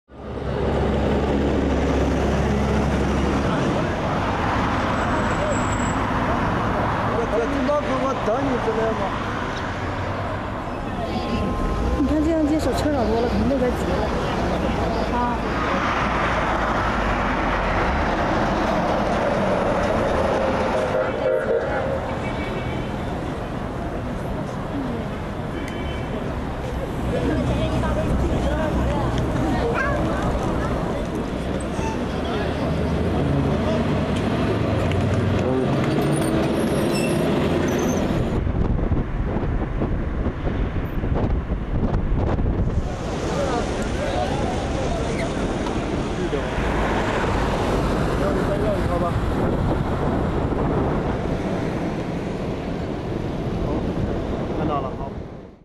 Tiananmen square car crash: three people die after car crashes in Chinese capital
Chinese state media say three people have died after a vehicle crashed in Tiananmen Square in the capital, Beijing.